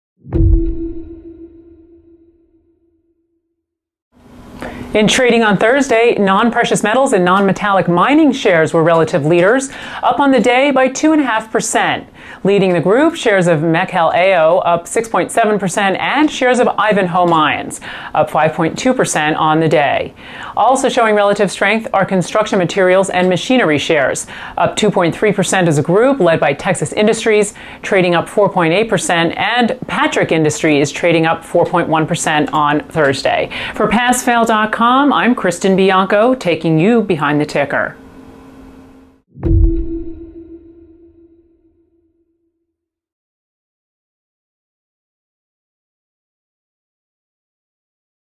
Passfail.com News: Thursday Sector Leaders: Non-Precious Metals & Non-Metallic M...
In trading on Thursday, non-precious metals & non-metallic mining shares were relative leaders, up on the day by about 2.5%.  Leading the group were shares of Mechel Oao (MTL), up about 6.7% and shares of Ivanhoe Mines Limited (IVN) up about 5.2% on the day.

Also showing relative strength are construction materials & machinery shares, up on the day by about 2.3% as a group, led by Texas Industries (TXI), trading up by about 4.8% and Patrick Industries (PATK), trading up by about 4.1% on Thursday.

This is Kristin Bianco for Passfail.com, taking you behind the ticker.